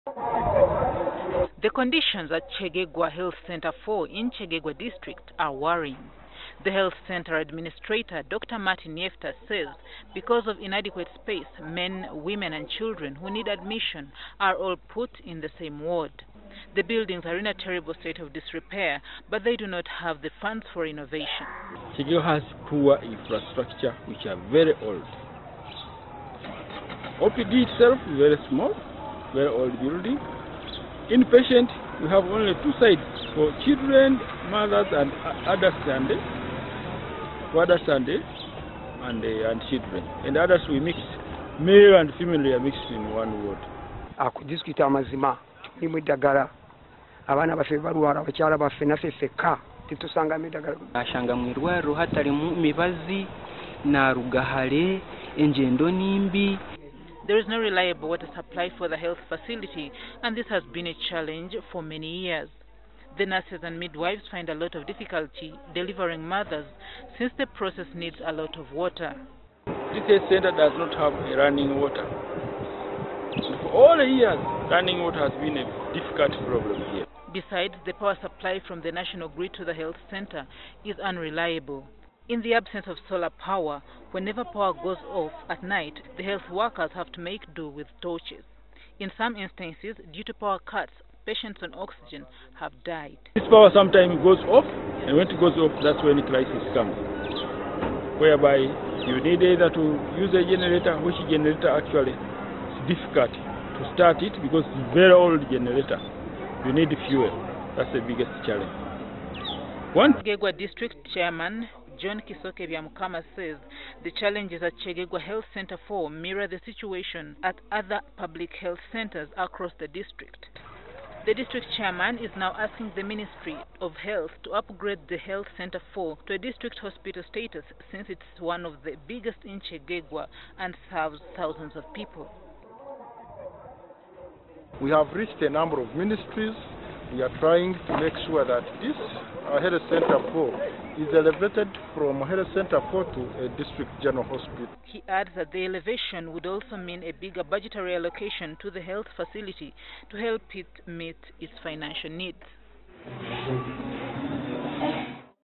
Kyegegwa Health Centre 4 operating under dire conditions
The dire state of Kyegegwa Heath Centre Four in Kyegegwa district in western Uganda is worrying thousands of residents served by the public facility. Due to space constraints, patients admitted to the facility are put in one ward; be they children, men and women. The Kyegegwa district chairman is now asking government to elevate the health centre to district hospital status so that it can receive a bigger budget to run operations and improve the infrastructure.